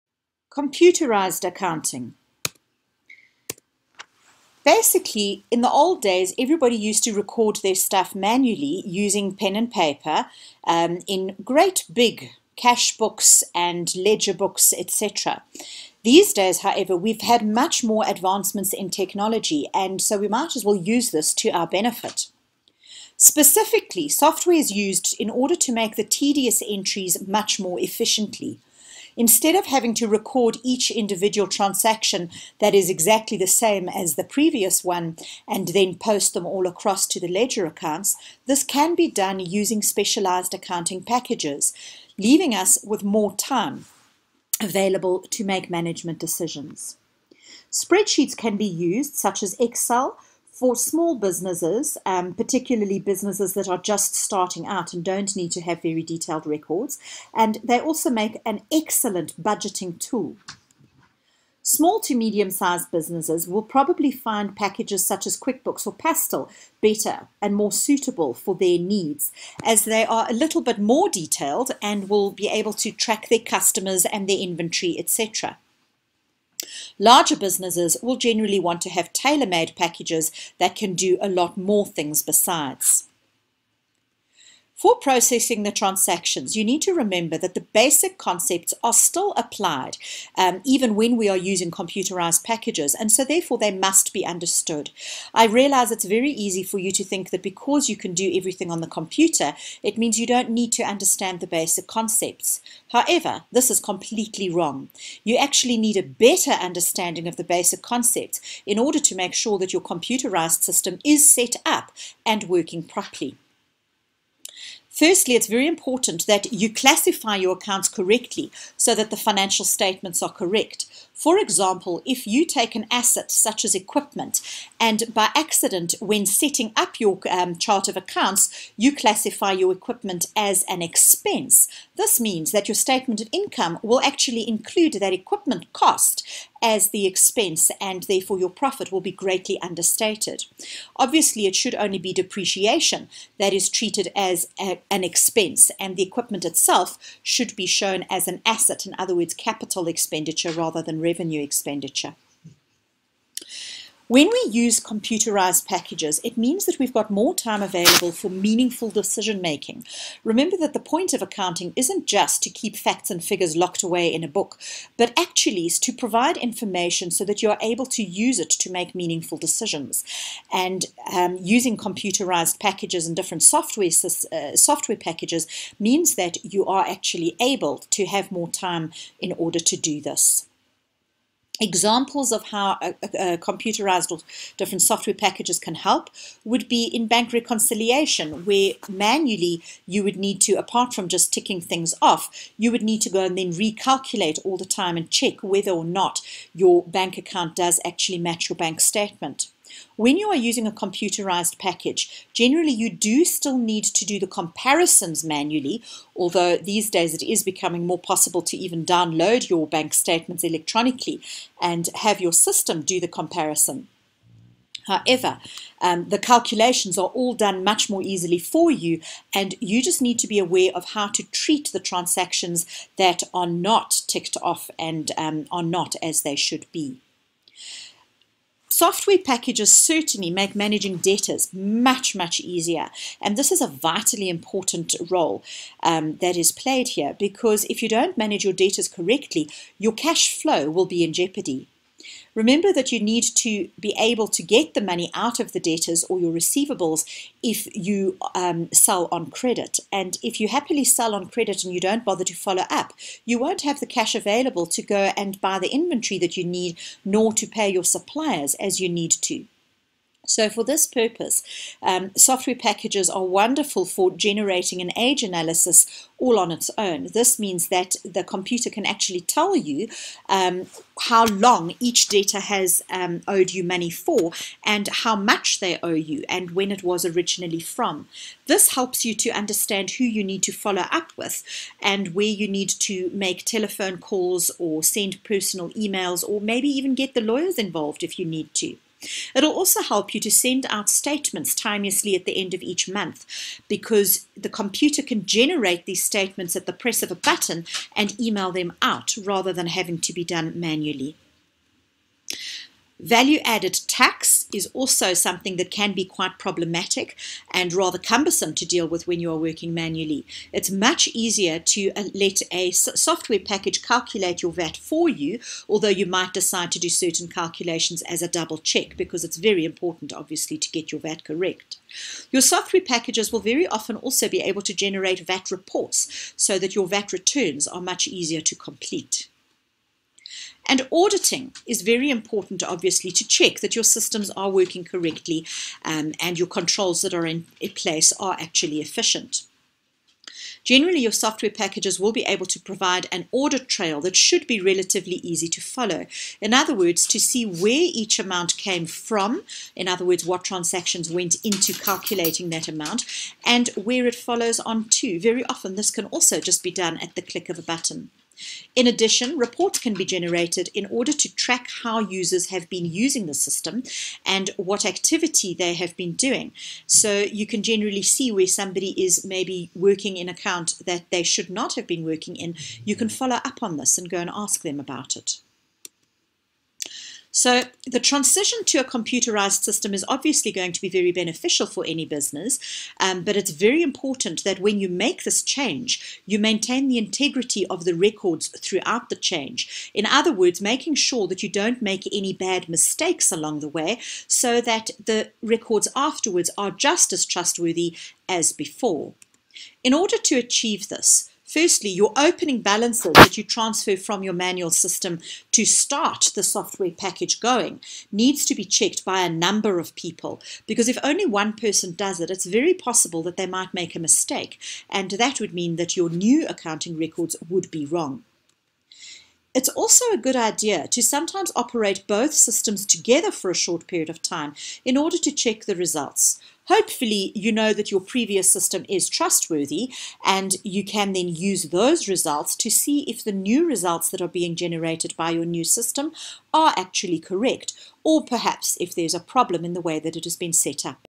1.4. Computerised accounting systems
An explanation of the use of computerised accounting systems.